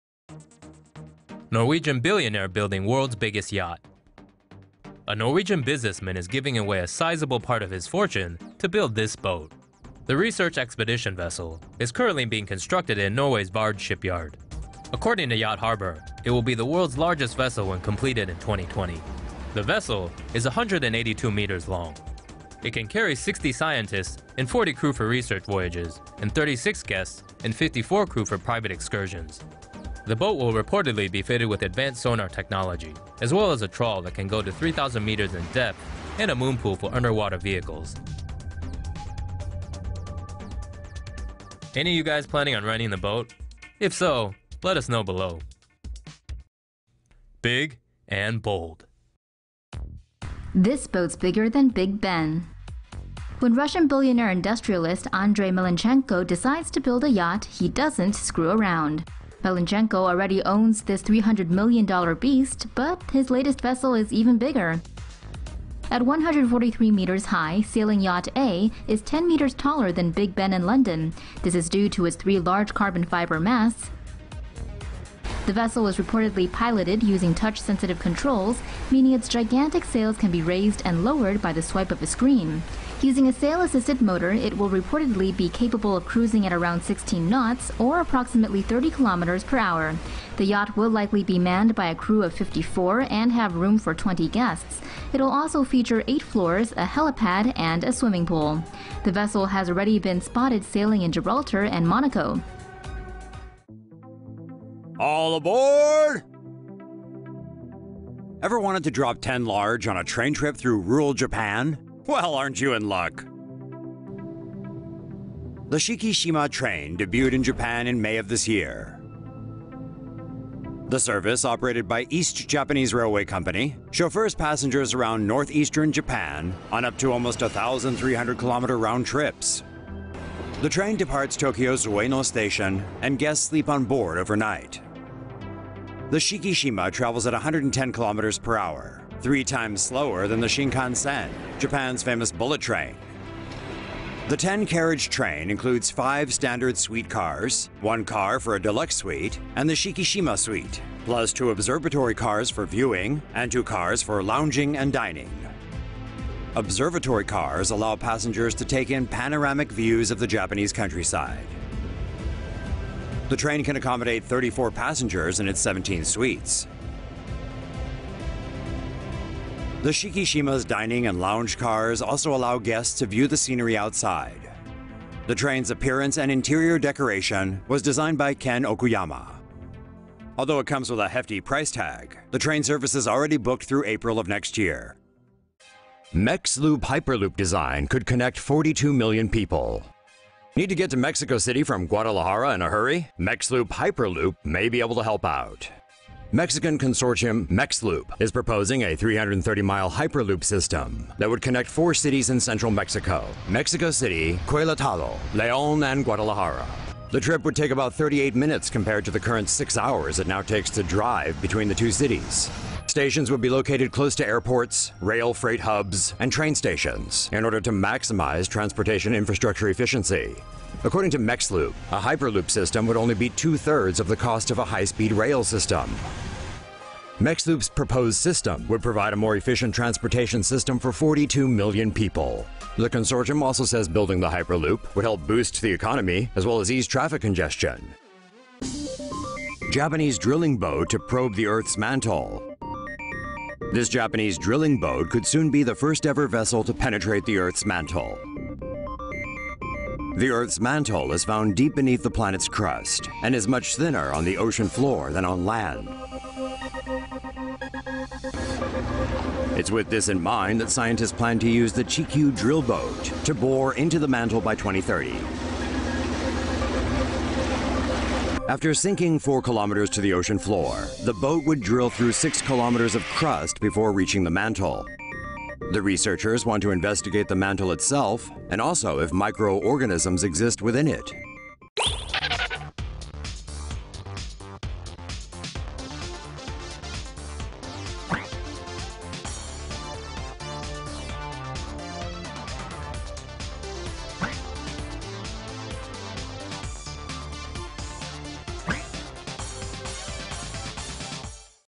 Biggest yacht in the world Norway REV yacht will be world's largest TomoNews
Norwegian businessman Kjell Inge Rokke is giving away a sizeable part of his fortune to build this boat.
Designed by Espen Oeino, the Research Expedition Vessel is currently being constructed in
Vard, Norway  According to Yacht Harbor, it will be the world's largest vessel when completed in 2020.
The boat is 182 meters long.  It can carry 60 scientists and 40 crew for research voyages, and 36 guests and 54 crew for private excursions.
According to its website, the boat will be fitted with advanced sonar technology, as well as a trawl that can go to 3000m in depth and a moonpool for underwater vehicles that can go to 6000m depth.